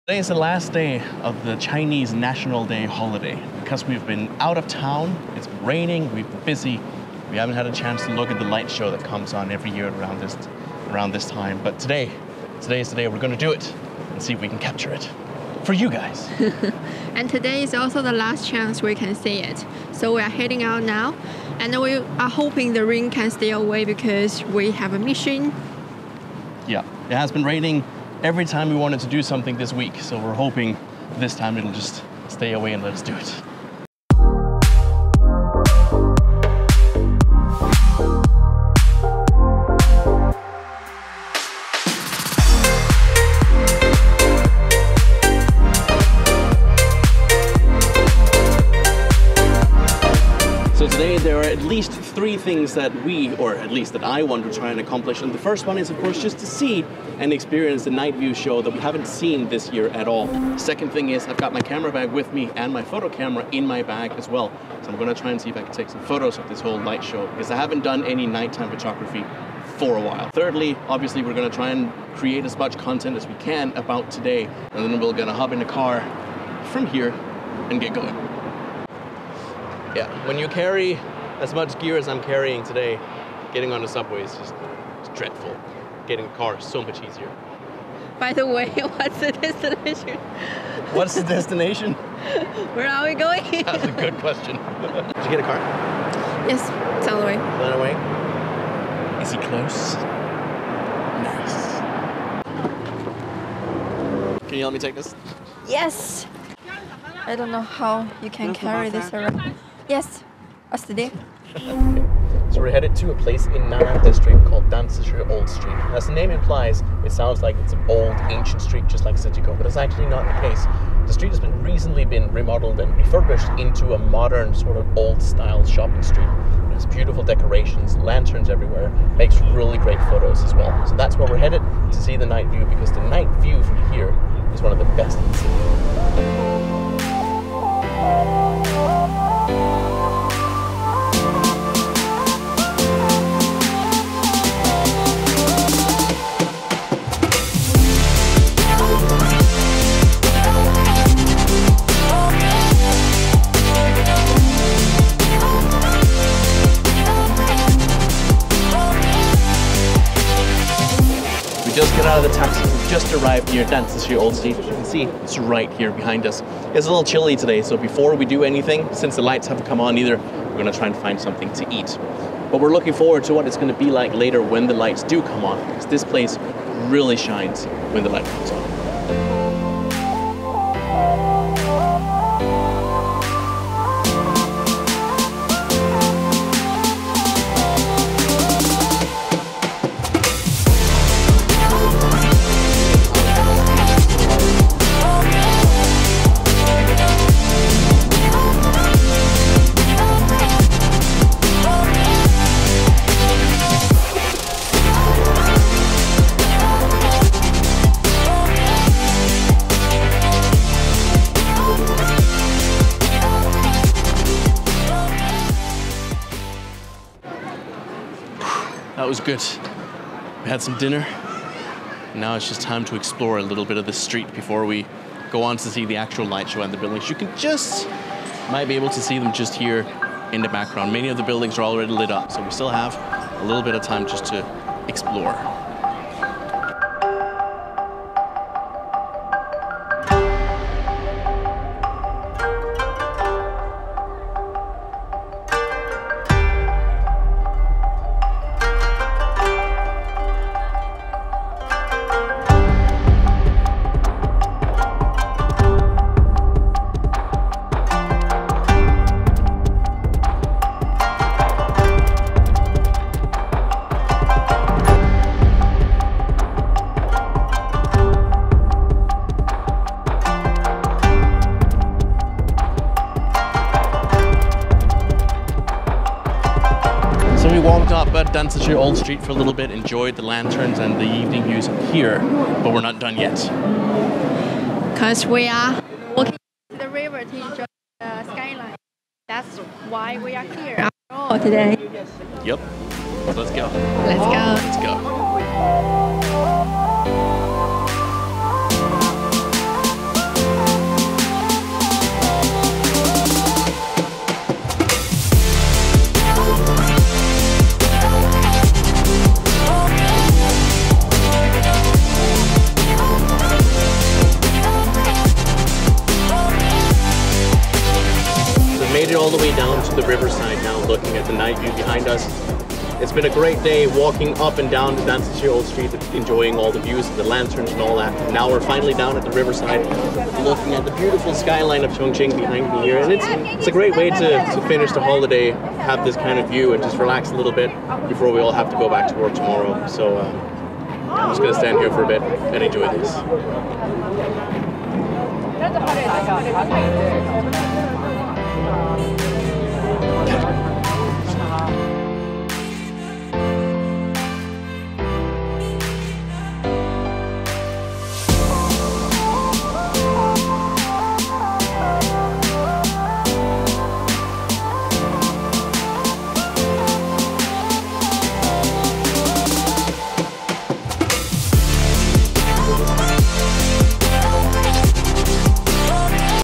Chongqing's Amazing Skyline | Mikkel's Vlog
On this episode of Mikkel's Vlog it is the last day of the Chinese National Day Holiday, and I am taking my wife out to make a video, and enjoy Chongqing's Amazing Skyline. The last day of the holiday was also the last night to enjoy the special light shows put on many of Chongqing's skyscrapers.